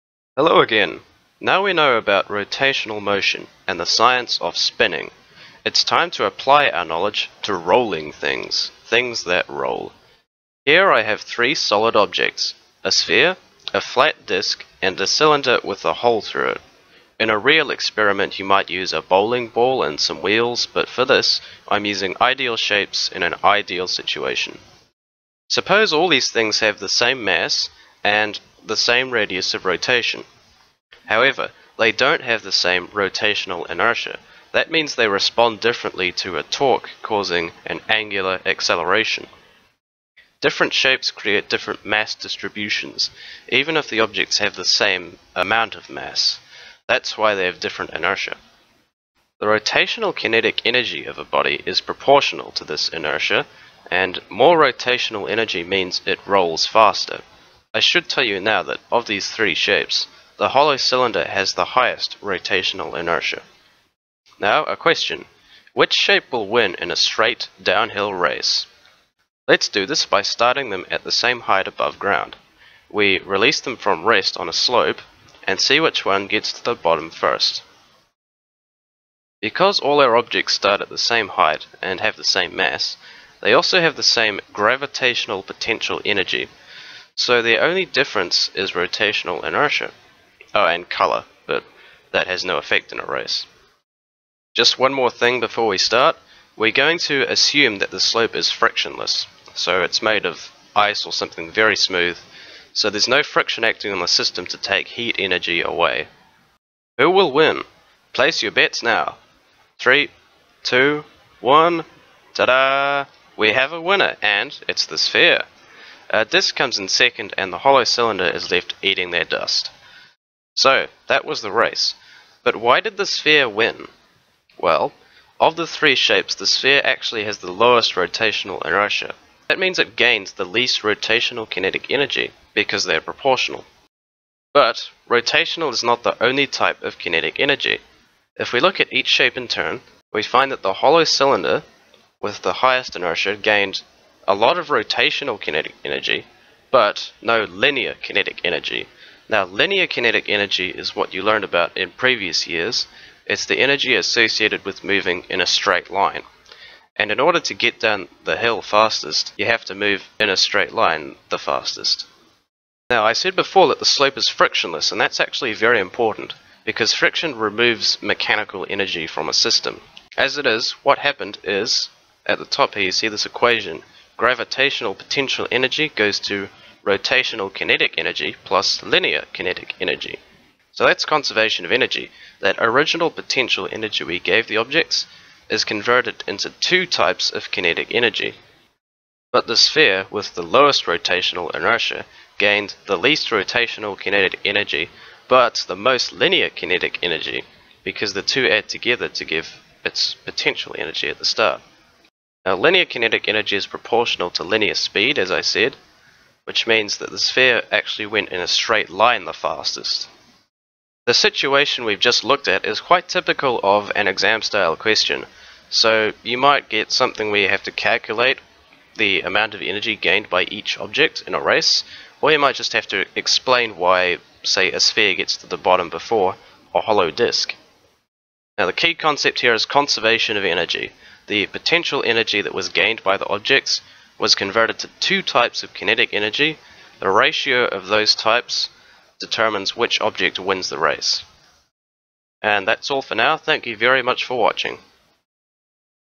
Conservation of Energy in Rolling Bodies
An explanation of the behaviours of differently shaped objects rolling down a hill. Emphasis is on the Law of Conservation of Energy, as well as rotational inertia and two types of kinetic energy.